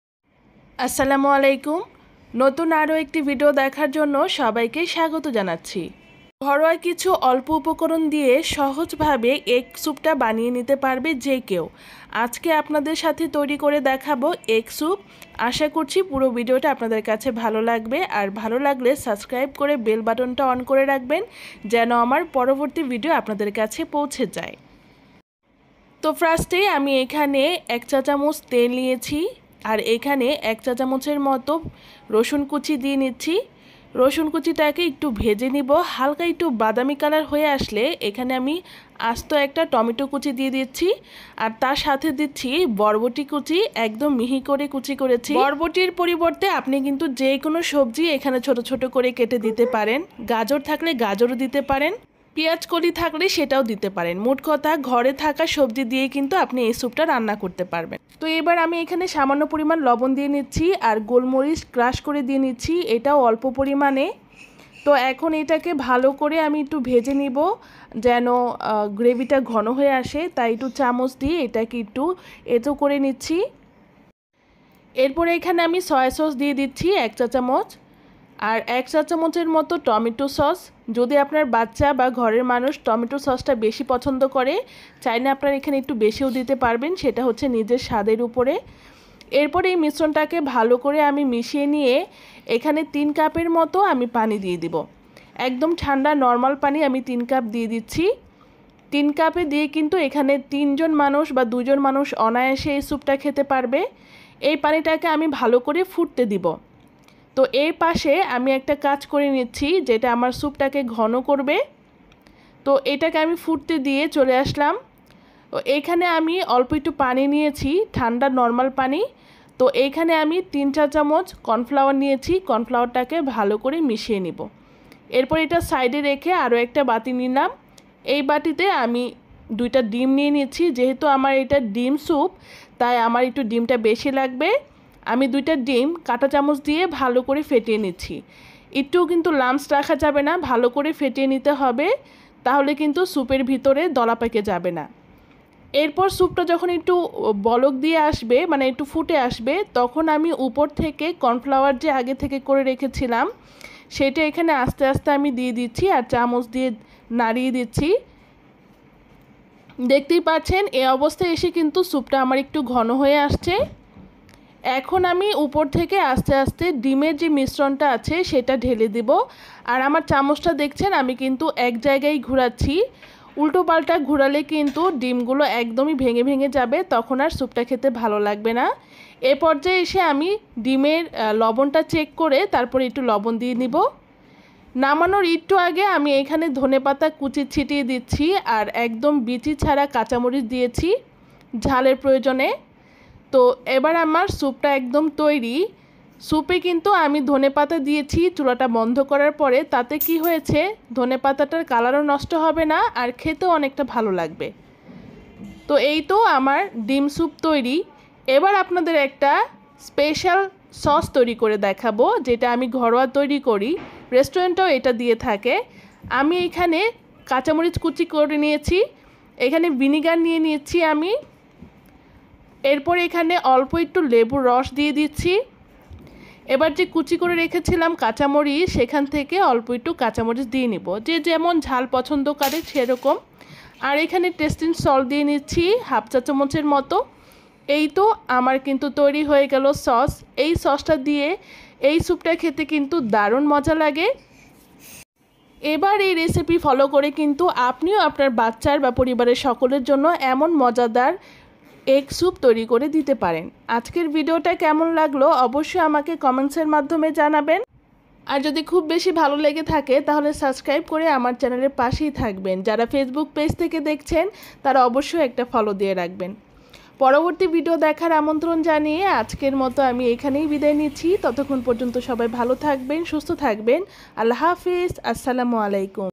খুব সহজে ঘরোয়া উপকরনে তৈরি ডিমের সুপ/ এগ ড্রপ সুপ | Egg Drop Soup | Tomato Egg Soup Recipe
| Egg Drop Soup | Tomato Egg Soup Recipe